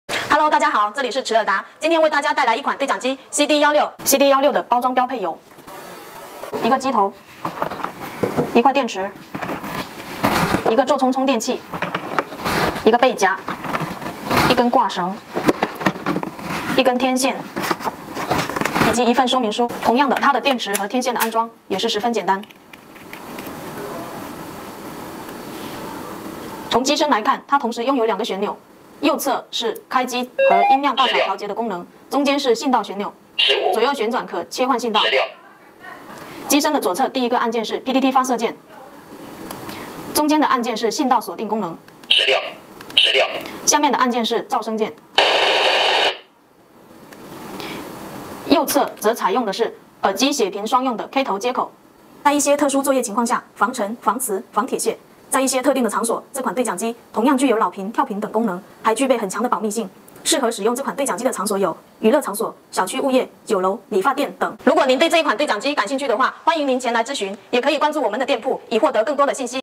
Chierda CD16：Decoding King and Standby King
CD16 Decoder King is more than just an ordinary walkie-talkie. Through its unique decoding technology, it can identify and parse out the special sub-tones hidden in the audio, taking you beyond your imagination. It is not only limited to decoding the audio of its own brand, it is also a passport that can decode the audio of other brands of machines. No matter which audio format it is, CD16 Decoder King can easily control it.